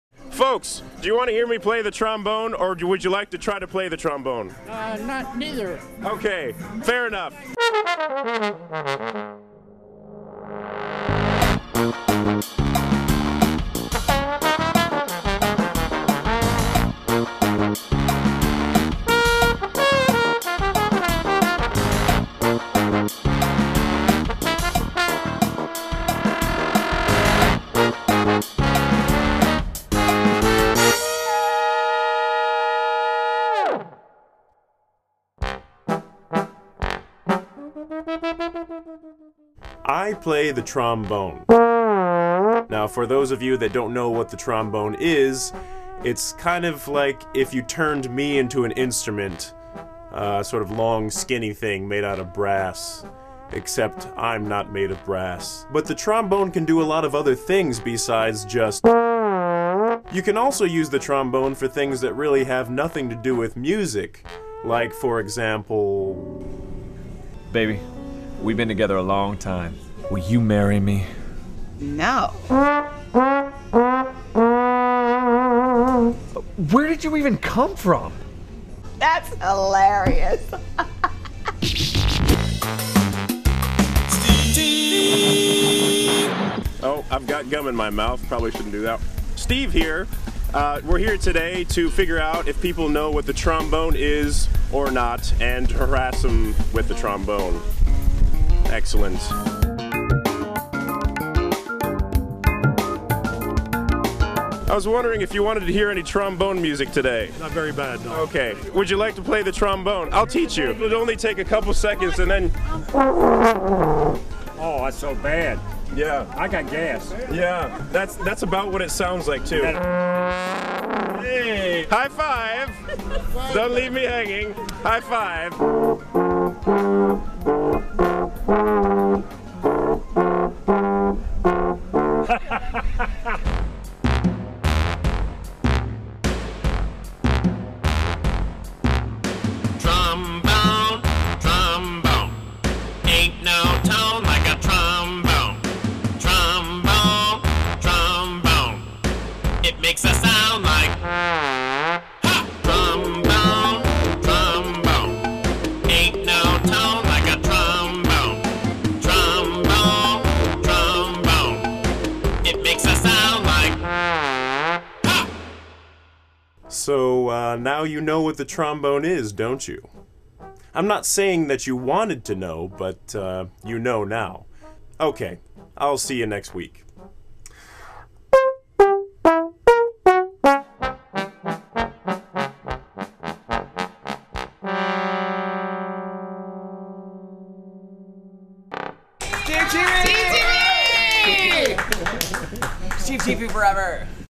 SteveTV: Trombone
Steve attempts to introduce the trombone to unwilling random people.